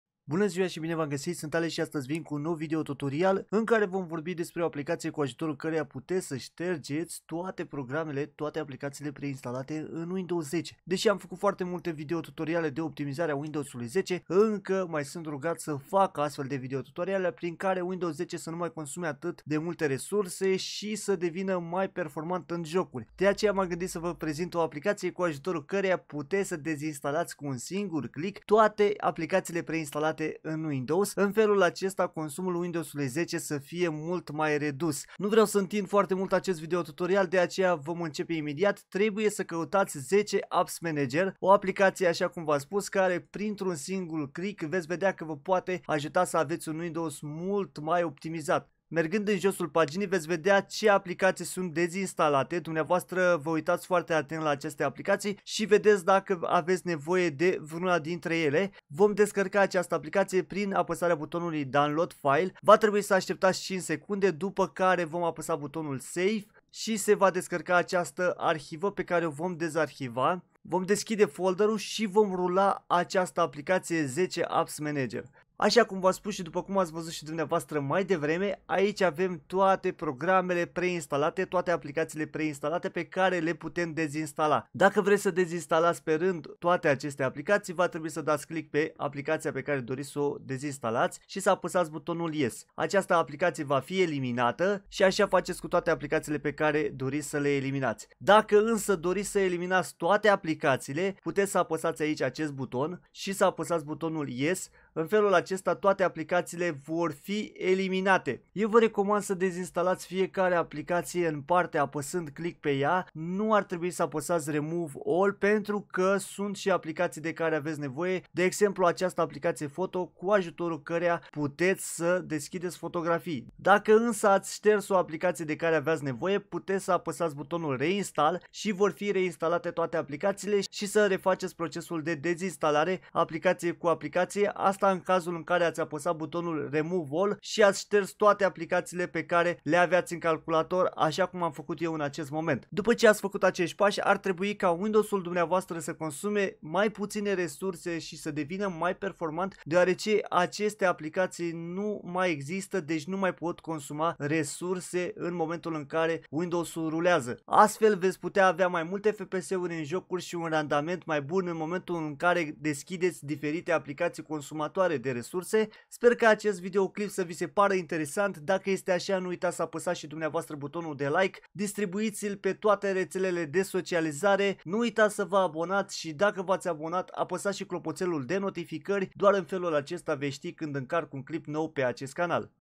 Windows 10 Optimizat Fara Aplicati Preinstalate
★  10AppsManager Aplicatia care te scapa de toate aplicatiile de care nu ai nevoie, aplicatii care consuma resurse si fac ca Windows-ul 10 sa consume resurse.
★ Donează aici minim 1 EURO ca să apari pe live (PayPal)